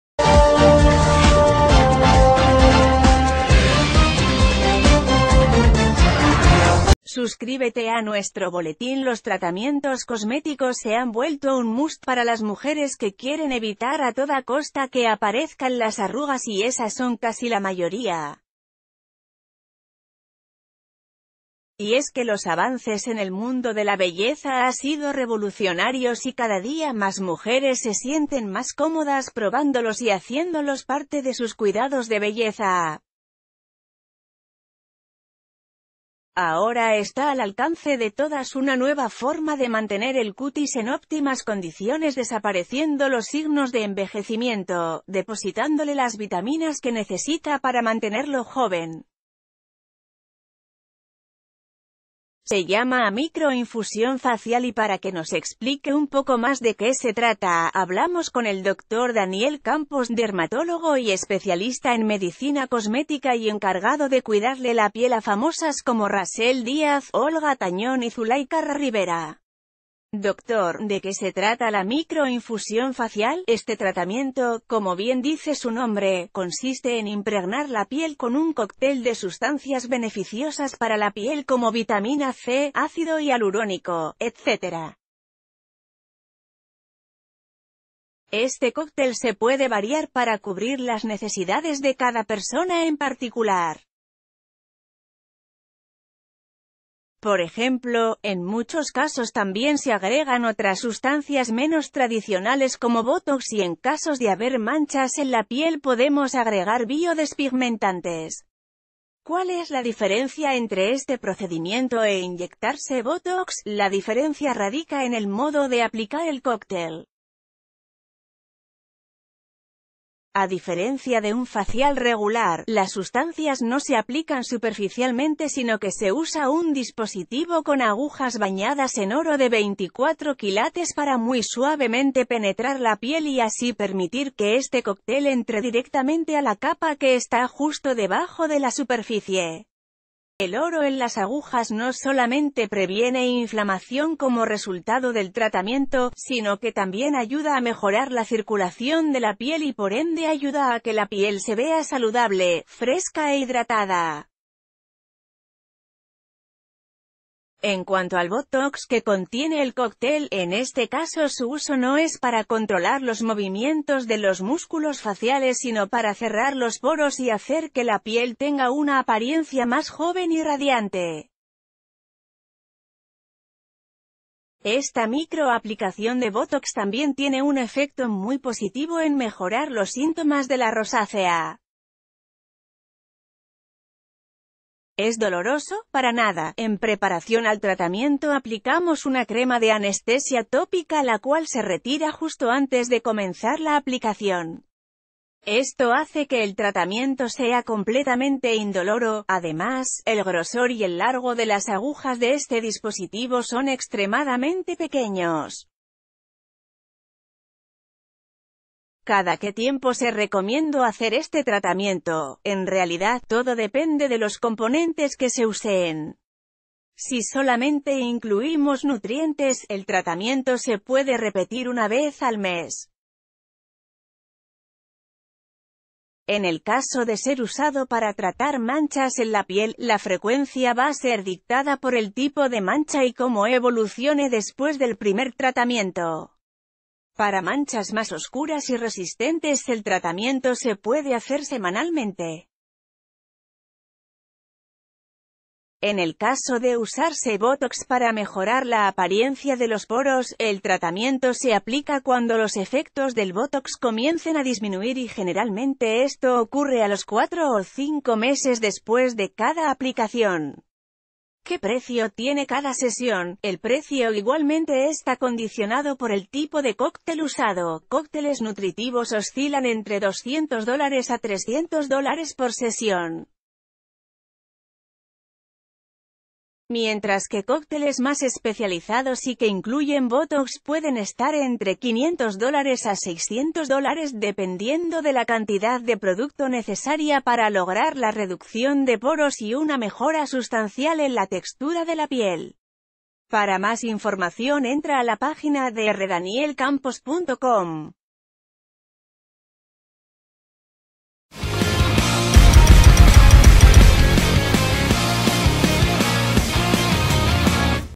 Mira de qué se trata el nuevo e innovador tratamiento facial favorito de las famosas
SUSCRÍBETE A NUESTRO BOLETÍN Los tratamientos cosméticos se han vuelto un must para las mujeres que quieren evitar a toda costa que aparezcan las arrugas y esas son casi la mayoría. Y es que los avances en el mundo de la belleza ha sido revolucionarios y cada día más mujeres se sienten más cómodas p...